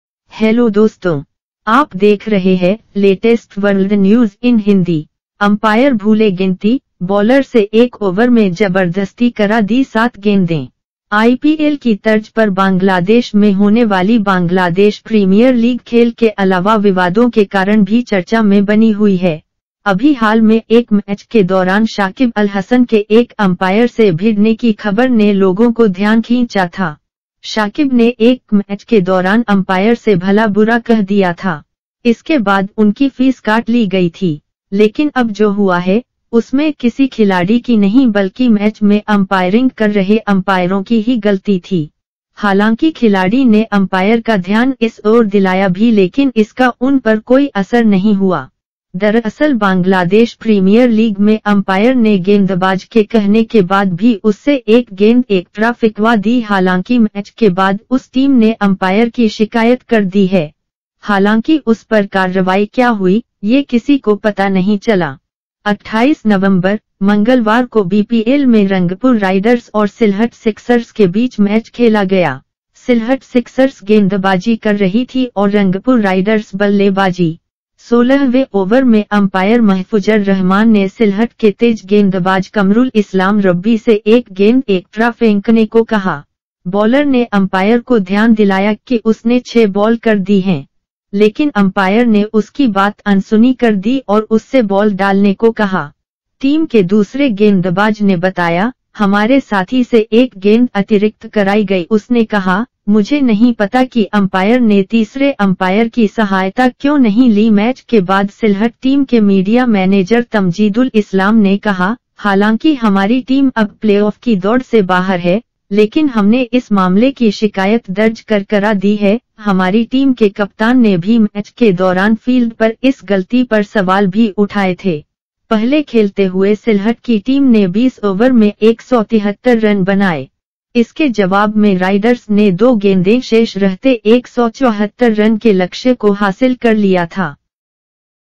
अंपायर भूले गिनती, बॉलर से एक ओवर में जबर्दस्ती करा दी 7 गेंदें ! Umpire foreget count during over
अंपायर भूले गिनती, बॉलर से एक ओवर में जबर्दस्ती करा दी 7 गेंदें ! Umpire foreget count during over in BPL
आईपीएल की तर्ज पर बांग्लादेश में होने वाली बांग्लादेश प्रीमियर लीग खेल के अलावा विवादों के कारण भी चर्चा में बनी हुई है. अभी हाल में एक मैच के दौरान शाकिब अल हसन के एक अंपायर से भिड़ने की खबर ने लोगों को ध्यान खींचा था. शाकिब ने एक मैच के दौरान अंपायर से भला बुरा कह दिया था. इसके बाद उनकी फीस काट ली गई थी. लेकिन अब जो हुआ है, उसमें किसी खिलाड़ी की नहीं बल्कि मैच में अंपायरिंग कर रहे अंपायरों की ही गलती थी. हालांकि खिलाड़ी ने अंपायर का ध्यान इस ओर दिलाया भी ,लेकिन इसका उन पर कोई असर नहीं हुआ.